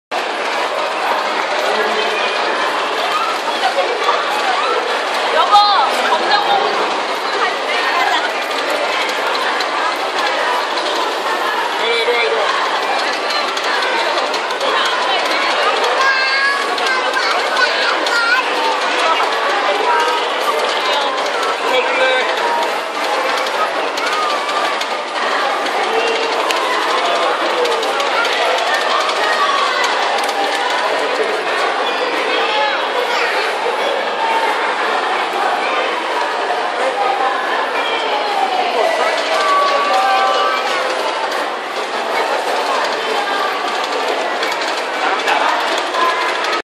[캐릭터 라이선싱 페어 2017] 검정고무신 기철이와 기영이
검정고무신의 기철 기영 형제 인형탈입니다